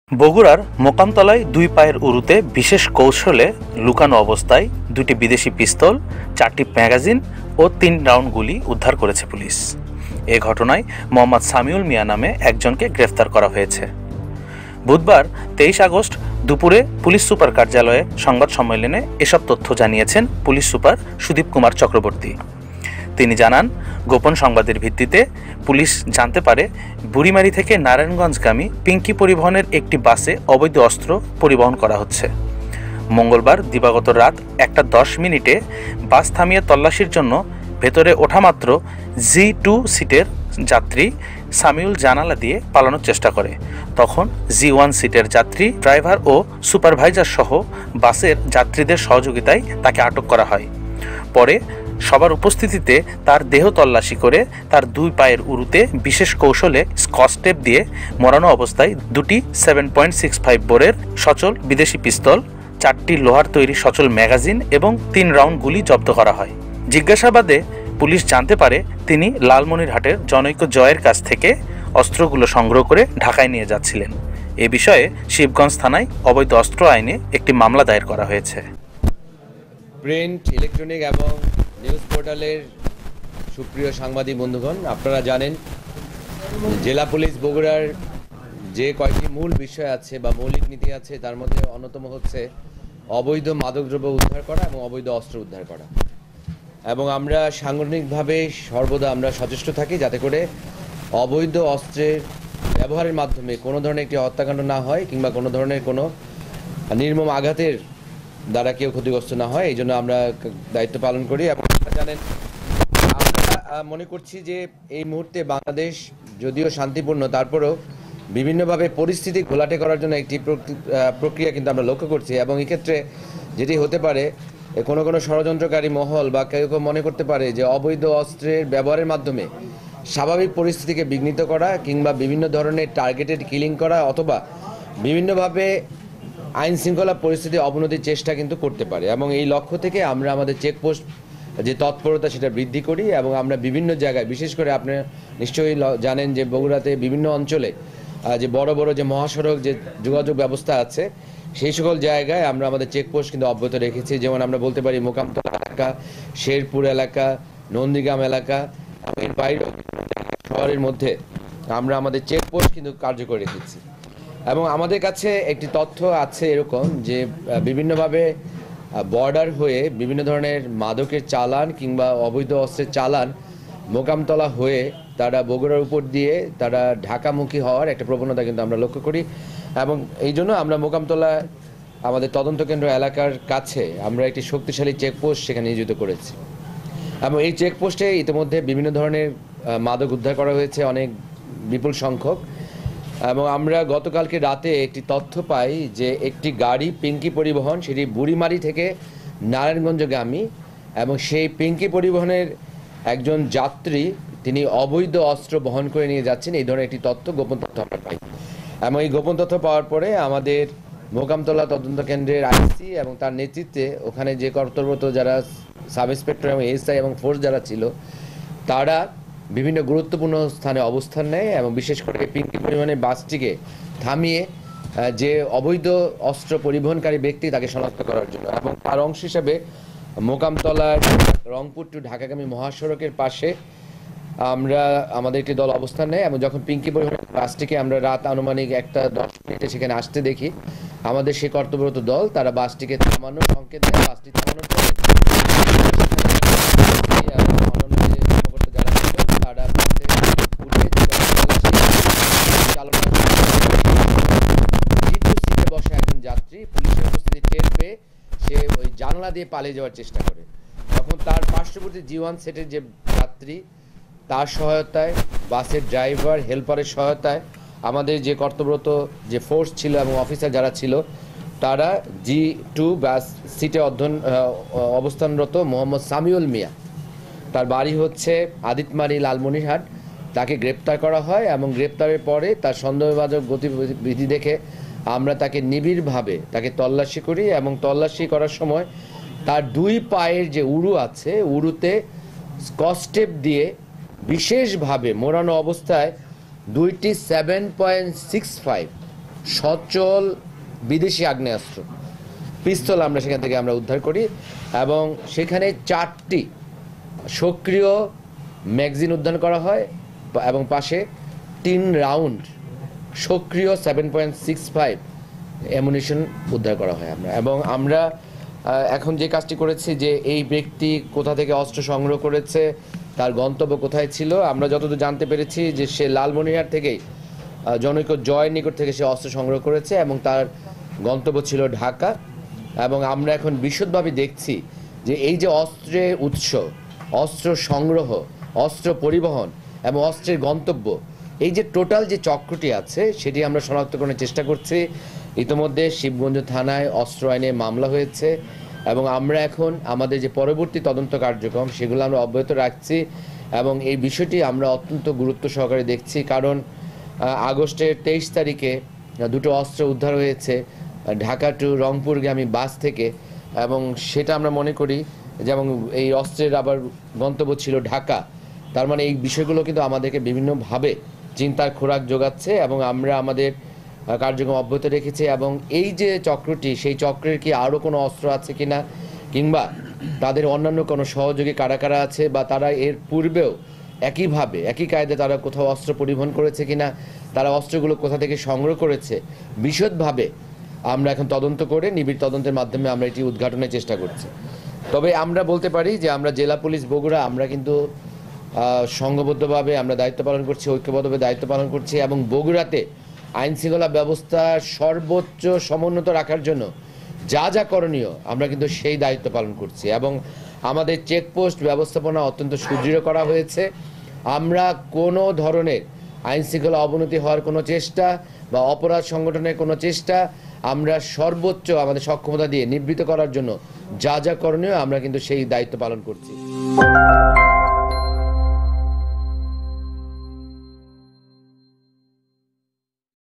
দুই পায়ের উরুতে বিশেষ কৌশলে অস্ত্র
দুই পায়ের উরুতে বিশেষ কৌশলে লুকানো অবস্থায় দুইটি বিদেশি পিস্তল, চারটি ম্যাগজিন ও তিন রাউন্ড গুলি উদ্ধার